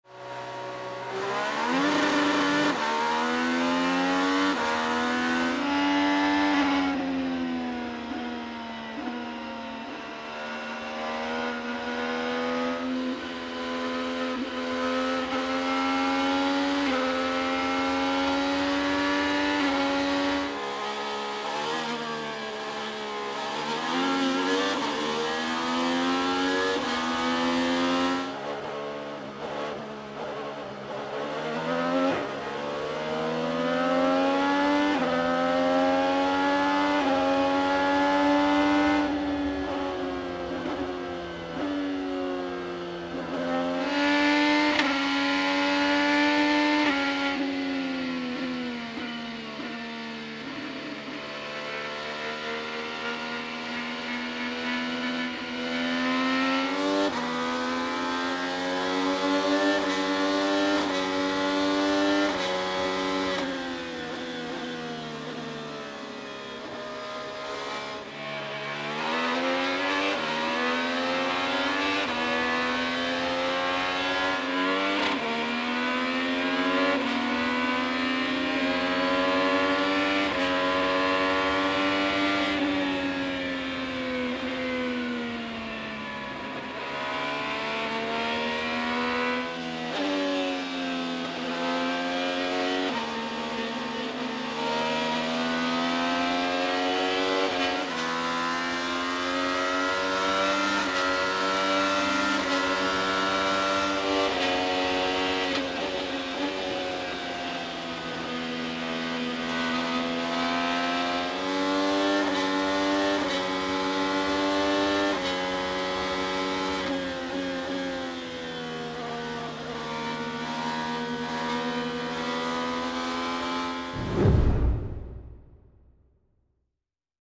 LAP 1 ONBOARDS // BOMMARITO AUTOMOTIVE GROUP 500
Nothing like wheel-to-wheel action on an oval. ⚔️

Go for a ride around World Wide Technology Raceway.